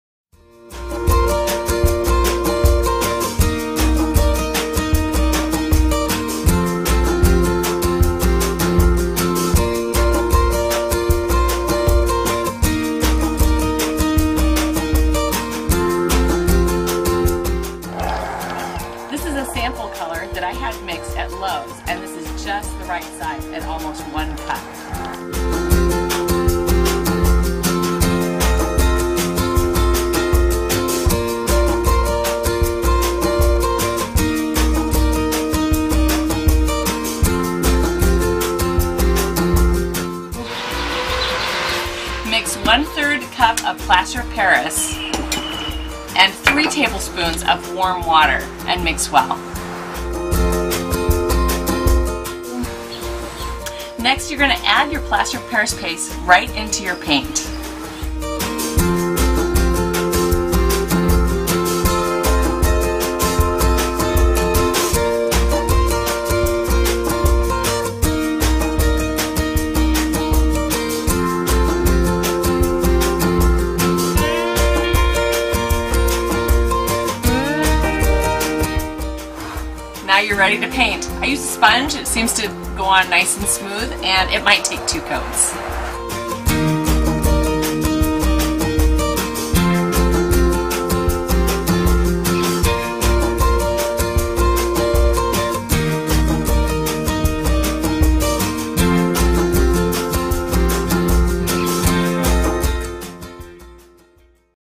DIY chalkboard paint
Making your own chalkboard paint is easy and the colors are limitless!